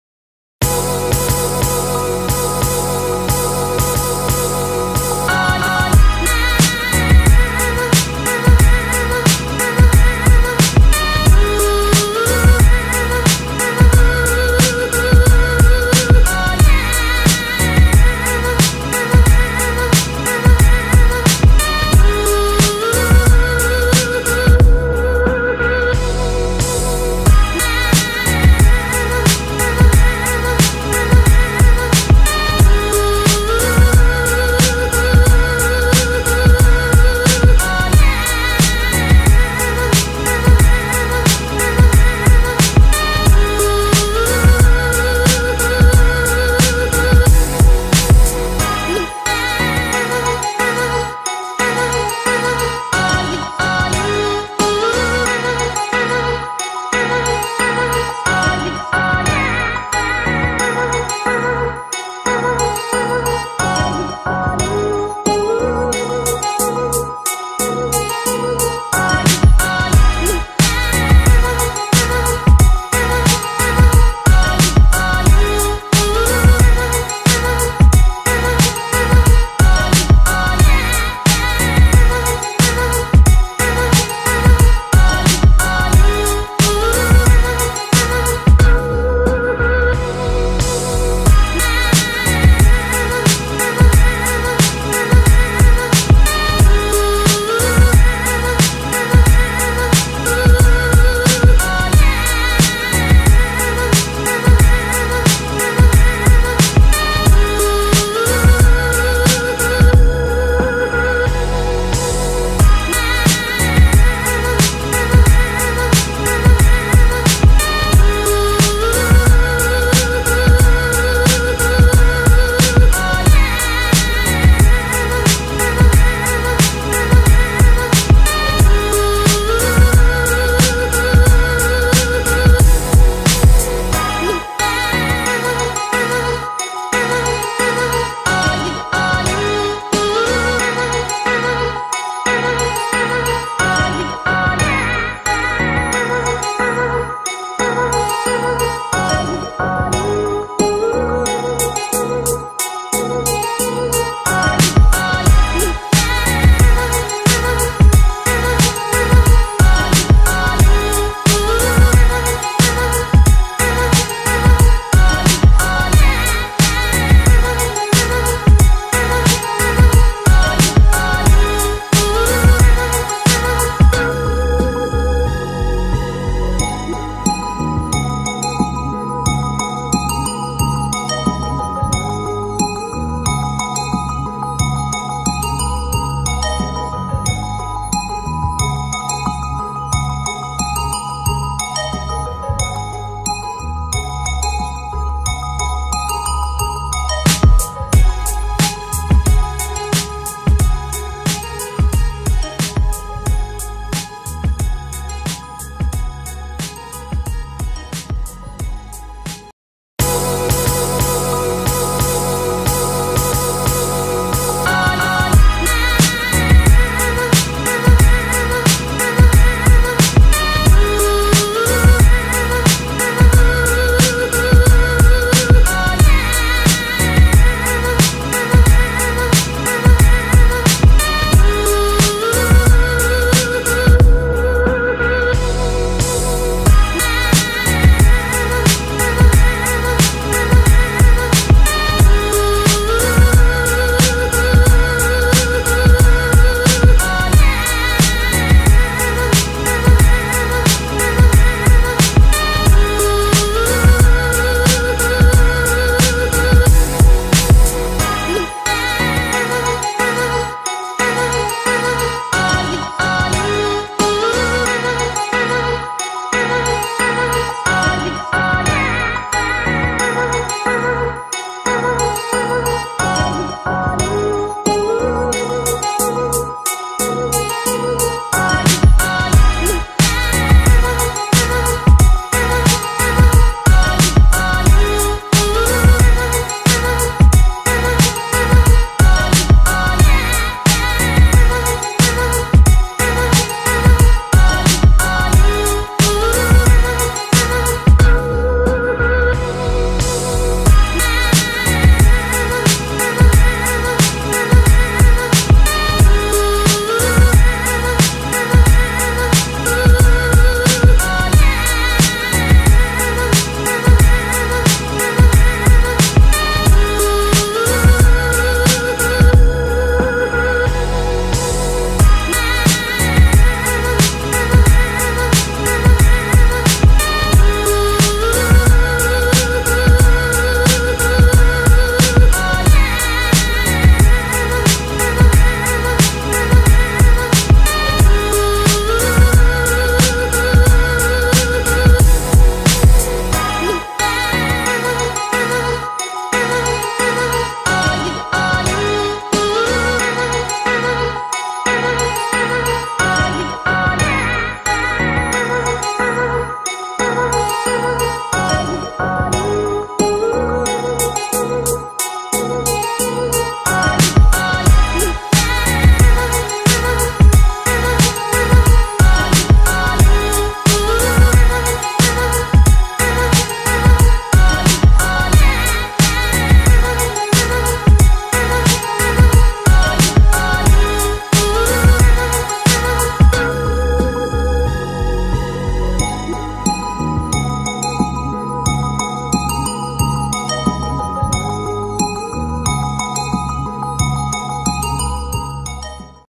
Dont worry about range on the Spektrum again
i instaled a 2 WAT signal booster in my DX6 becouse the stock range with a spek. satelite was  about 200-300 meters.in the video i tested it and the results are wery good
Booster i buyed -